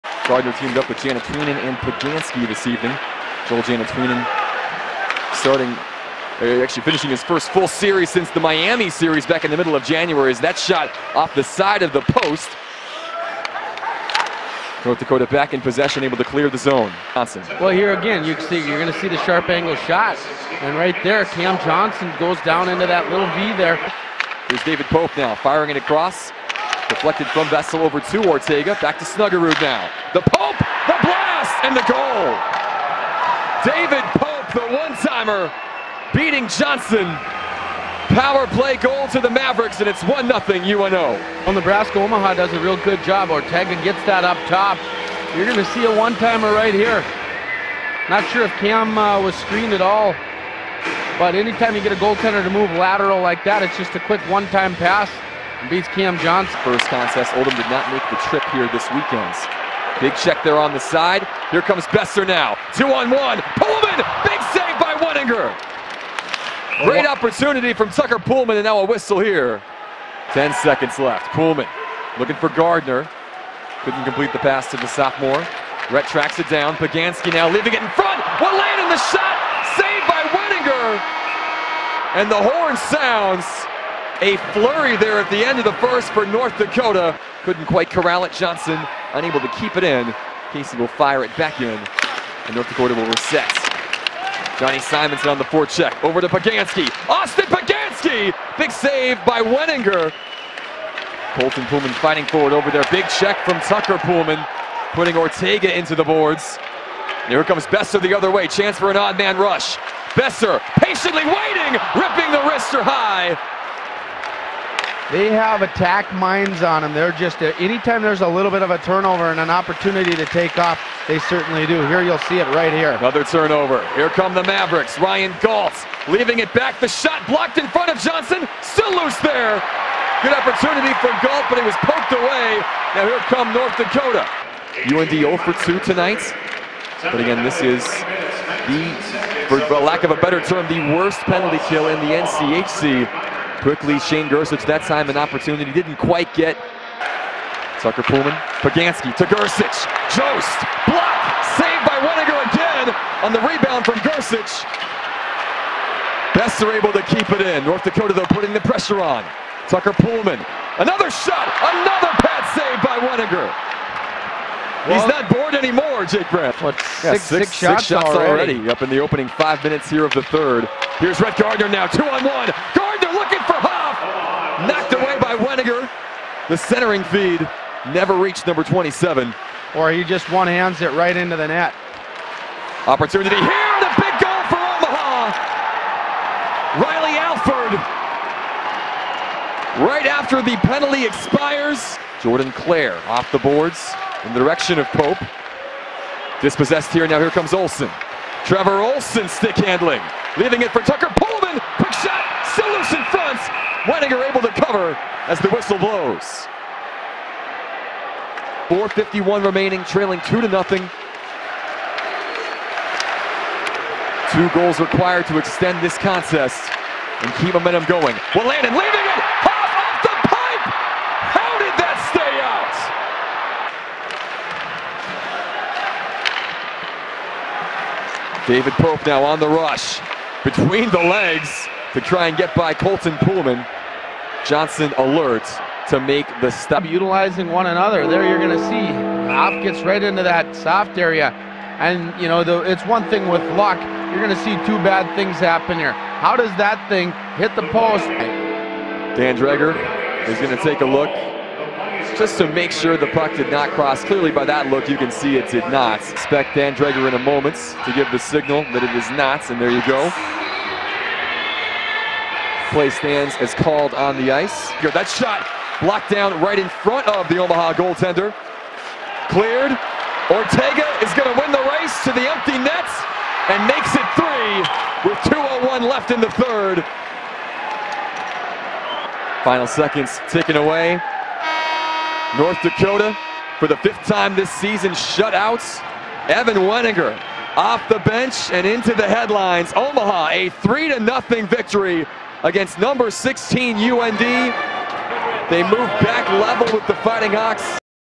UND hockey - Highlights vs Omaha - 2/25/17
Highlights between Omaha and the University of North Dakota, as the Mavericks beat the Fighting Hawks 3-0 in NCHC action on Saturday night at Ralph Engelstad Arena.